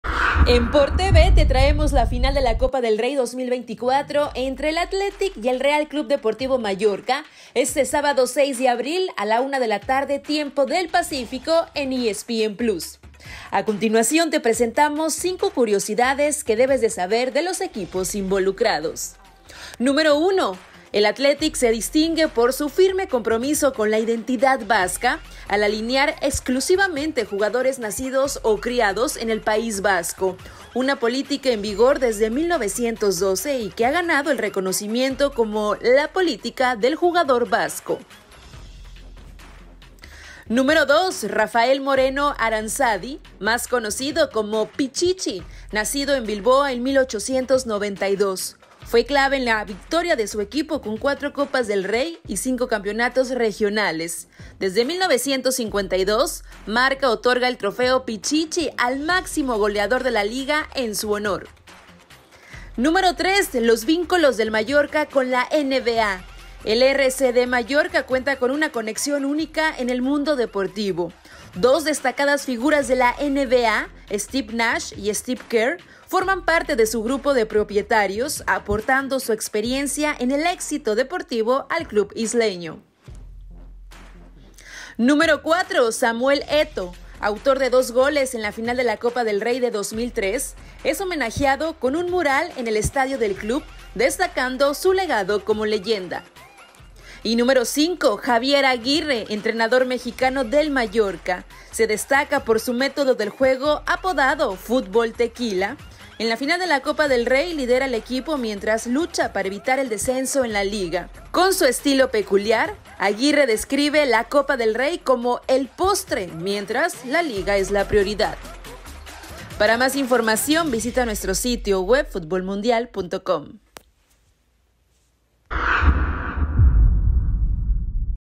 Copa del Rey detalles que no sabías de esta final, asi cómo dónde verla
En Por TV te traemos la final de la Copa del Rey 2024 entre el Athletic y el Real Club Deportivo Mallorca este sábado 6 de abril a las 1:00 pm PT en ESPN Plus!

A continuación te presentamos cinco curiosidades que debes de saber de los equipos involucrados: